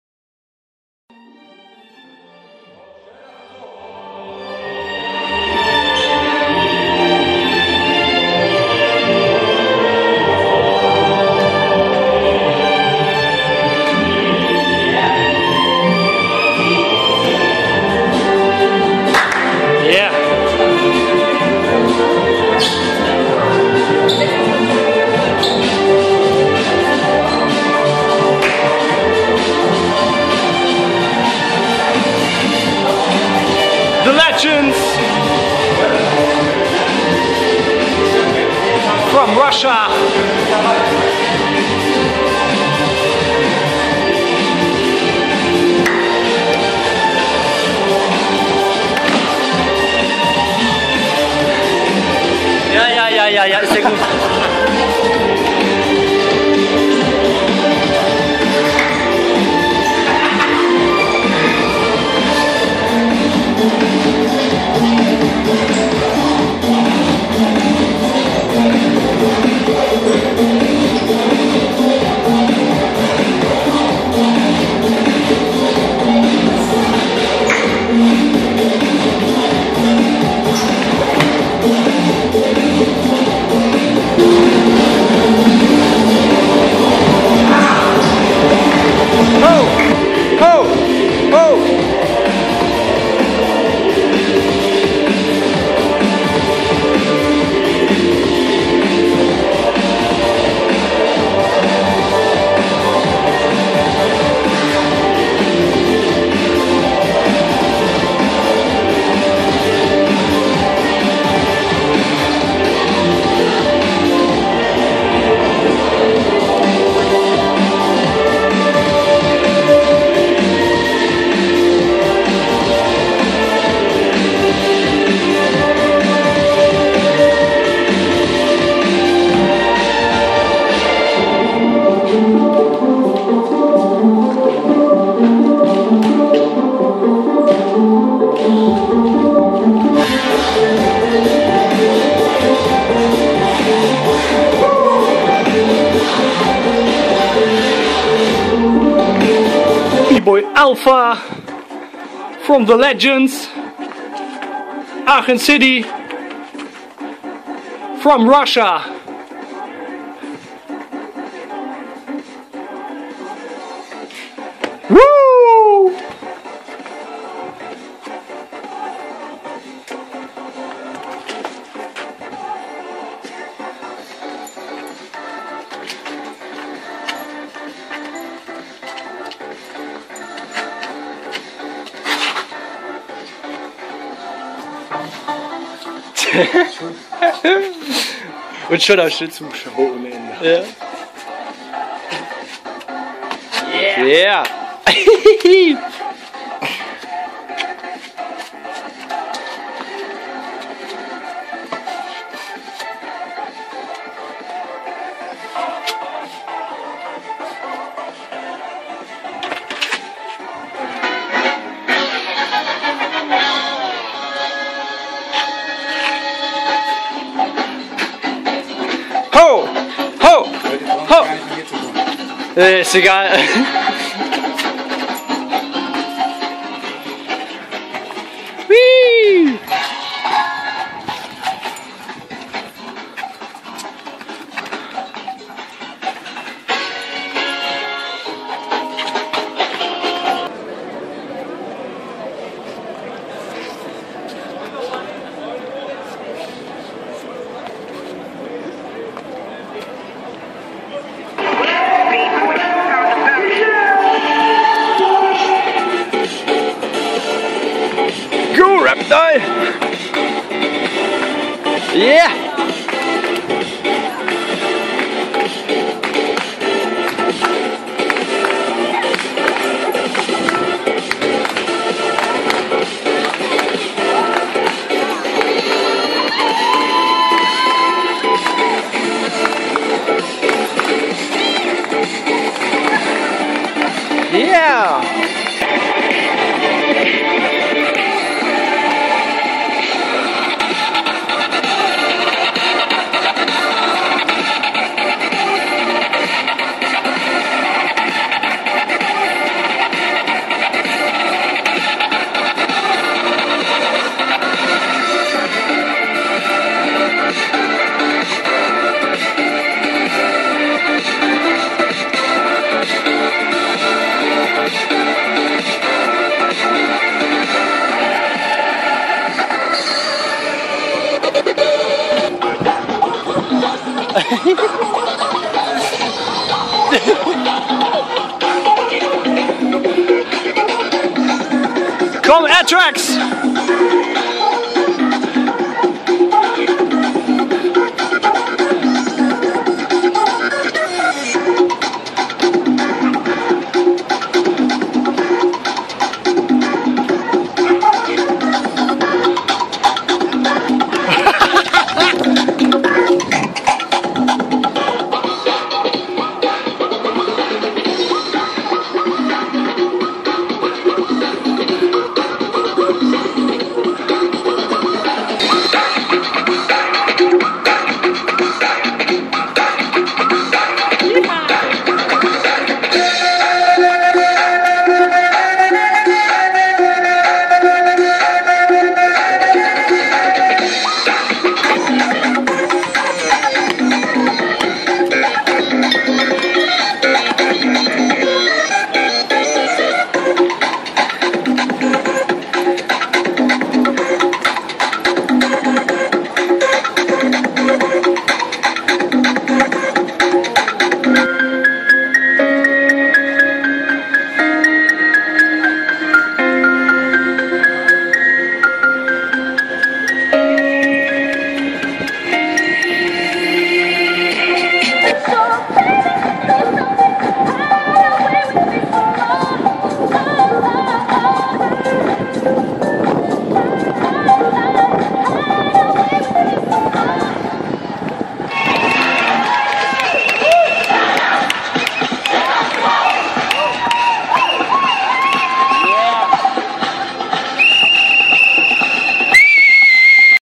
The Legends bei Bboy RuffBrawl in Köln - Old School Dance Session 2015
Bboy Alpha & Bboy Reptile machen Dance Day in Köln